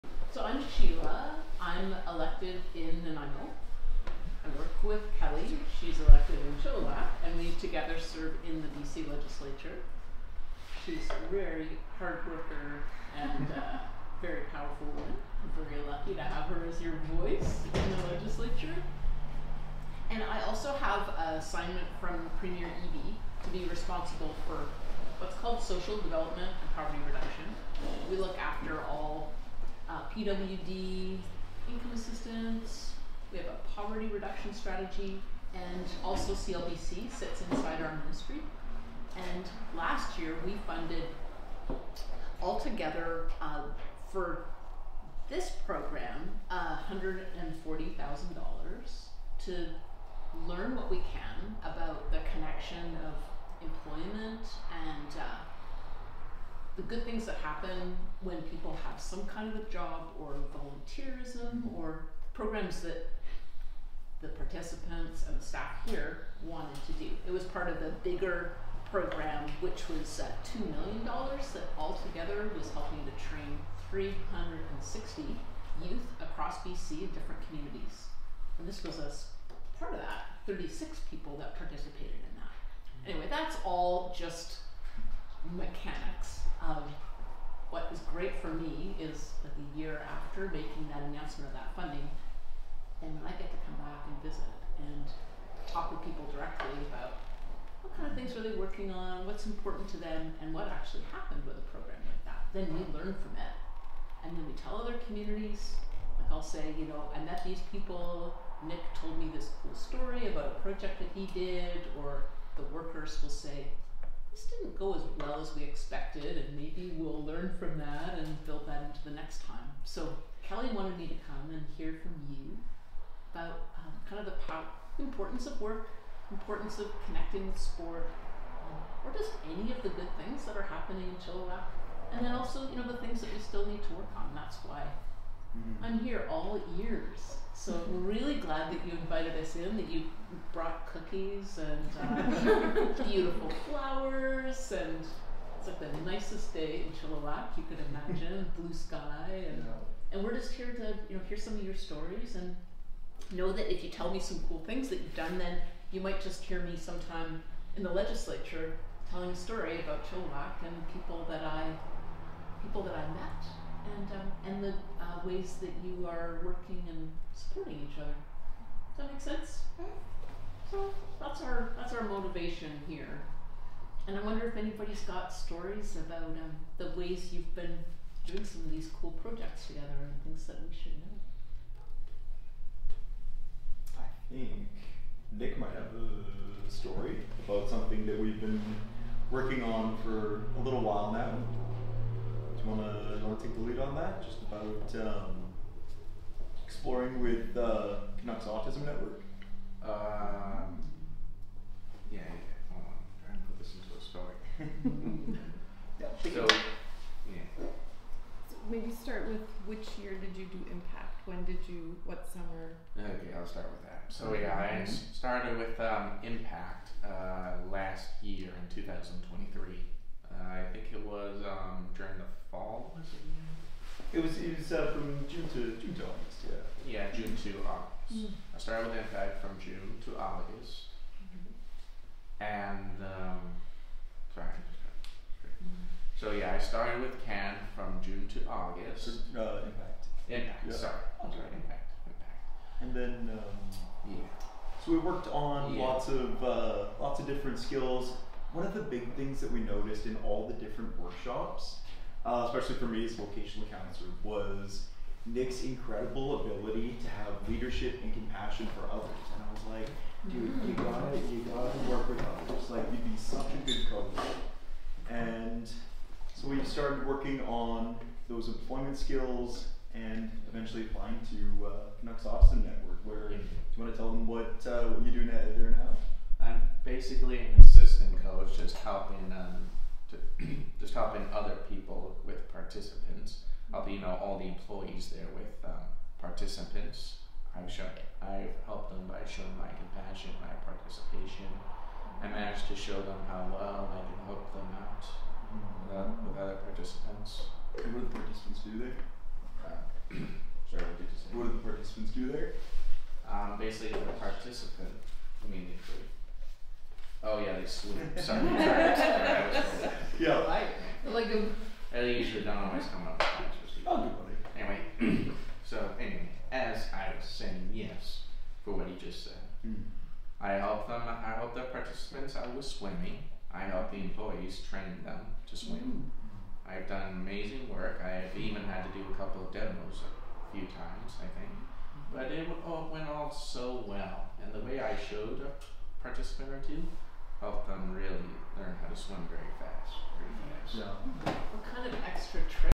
2024 CSCL Provincial Funding for Youth Programs to Secure Work for Youth April 19
BC Minister of Social Development and Poverty Reduction, Sheila Malcomson (foreground) Chilliwack-Kent MLA Kelli Paddon (green blazer) and participants and staff from CSCL Chilliwack Society for Community Living